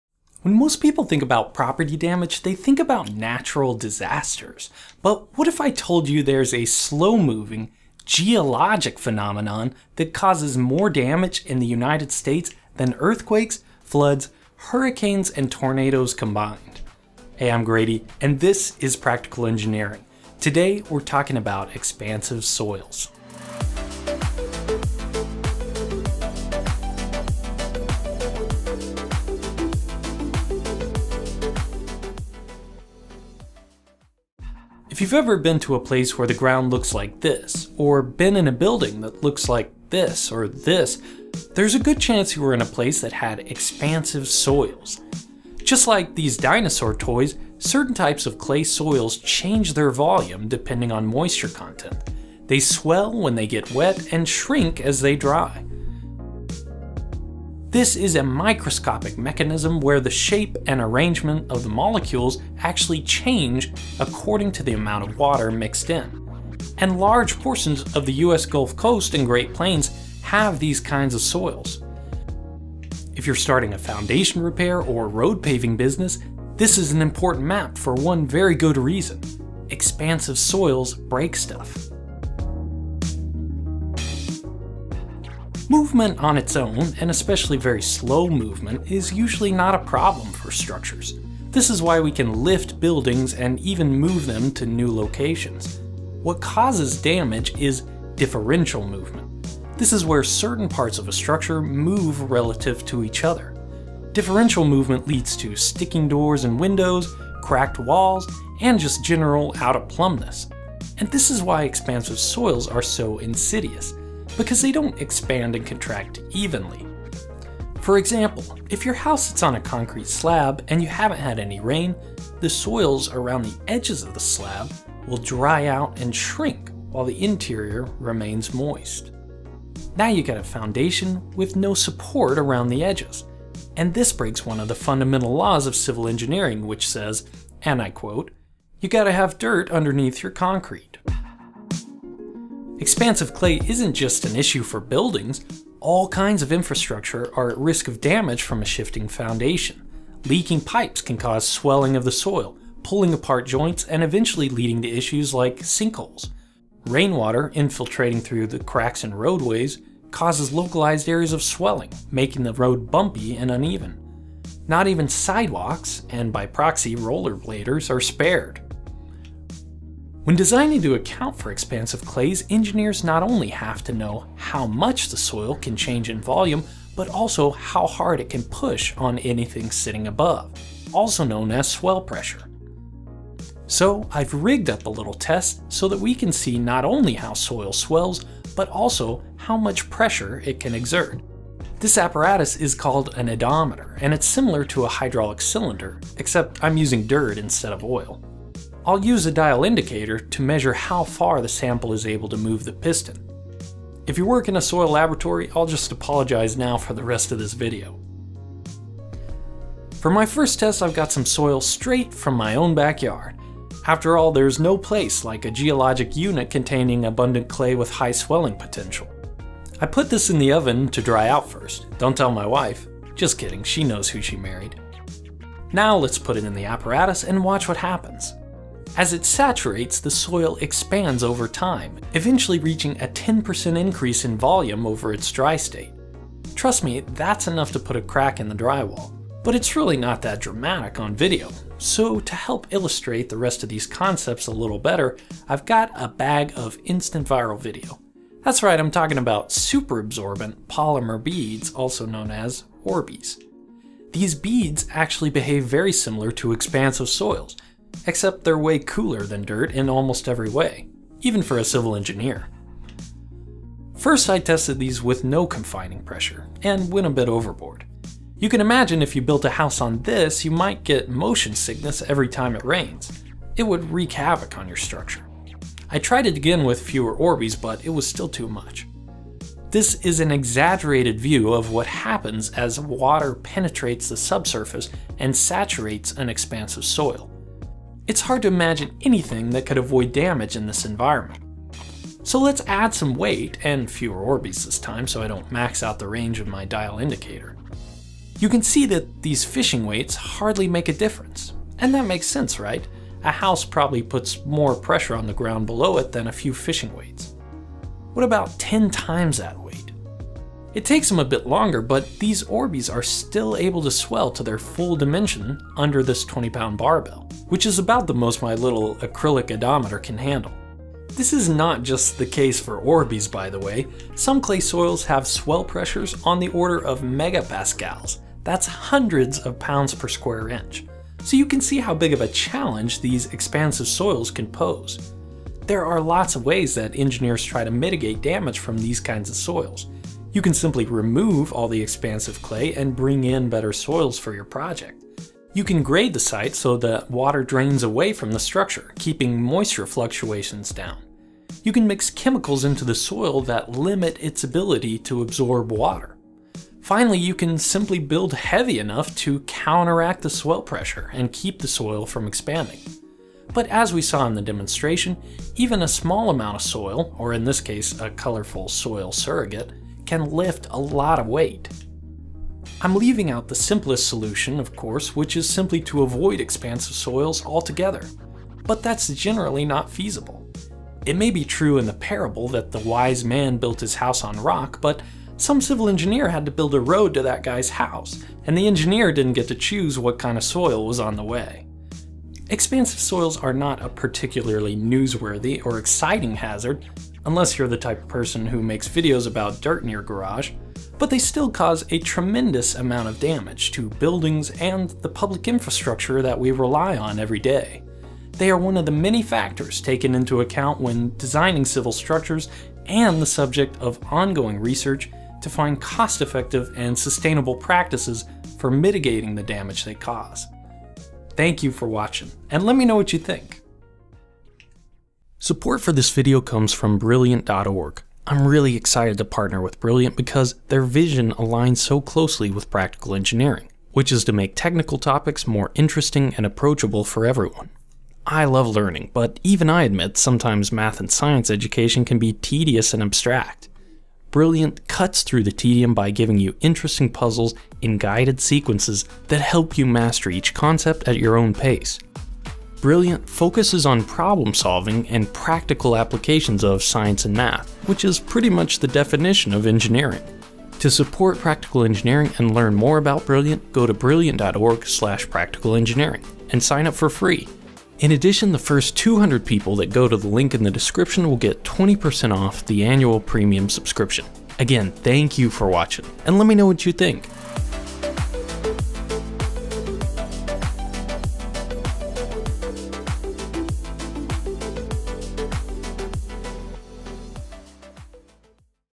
How Soil Destroys Buildings
Okay this is the last video on the hazards of soil mechanics for a while :)

Expansive soils cause more property damage per year than earthquakes, floods, hurricanes, and tornadoes combined. Expansive soils are a slow-moving geologic phenomenon, which makes them not very news-worthy. However, they still cause a tremendous amount of damage to buildings and the public infrastructure we rely on every day.

Watch this video and the entire Practical Engineering catalog ad-free on Nebula